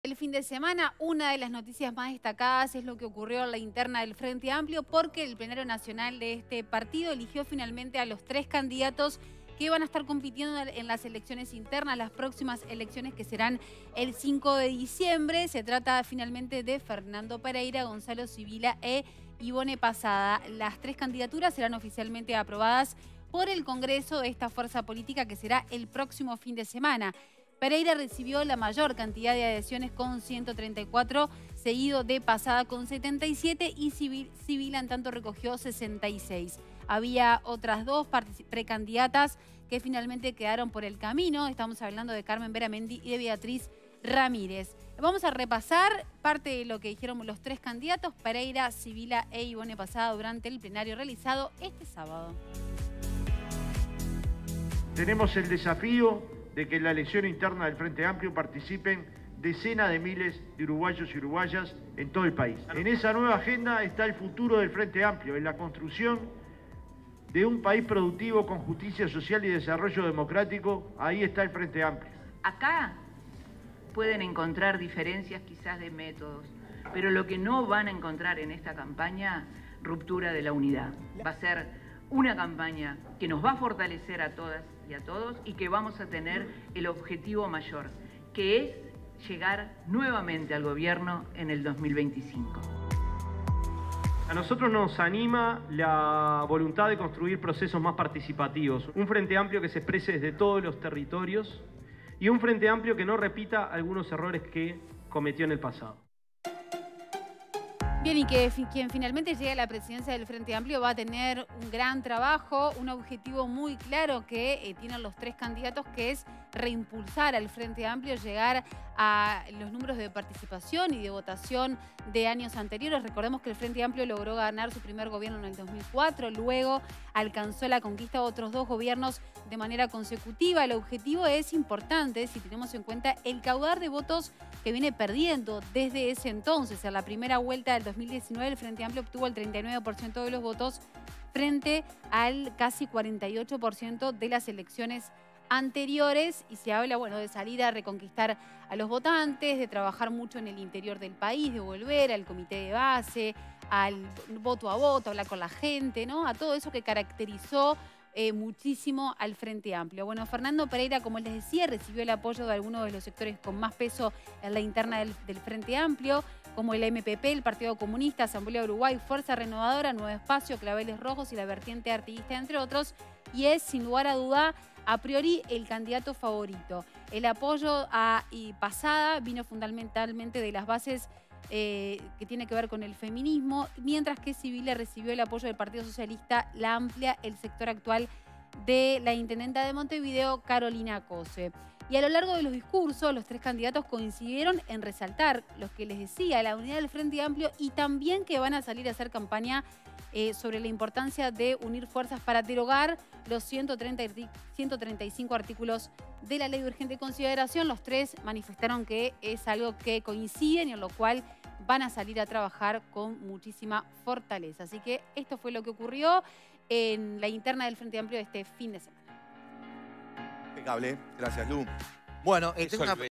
El Frente Amplio definió a sus tres precandidatos para la presidencia del partido
El plenario nacional eligió quiénes competirán por el liderazgo del partido de cara a las próximas elecciones del 5 de diciembre: Fernando Pereira, Gonzalo Civila e Ivonne Passada. Pereira recibió la mayor cantidad de adhesiones con 134, seguido de Passada con 77 y Civila recogió 66. Carmen Beramendi y Beatriz Rodríguez quedaron fuera de la carrera presidencial en el partido de izquierda.